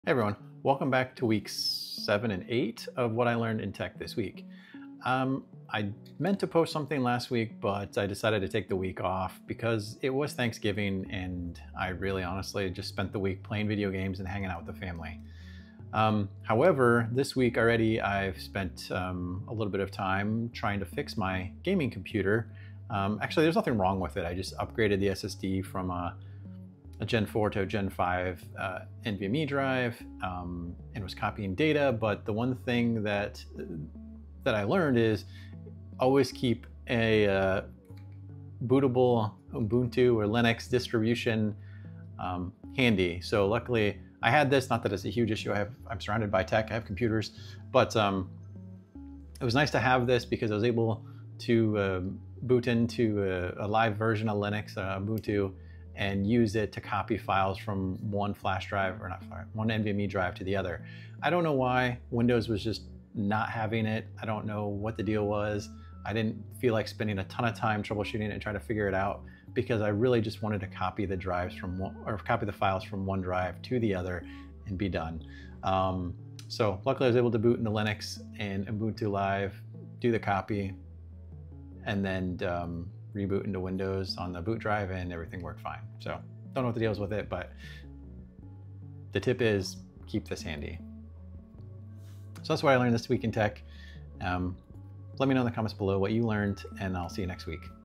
What I learned this week in Tech - Always keep a bootable linux live usb drive handy!
This week in tech i learned to always keep a bootable linux live usb drive handy for those times when windows doesn't want to play nice!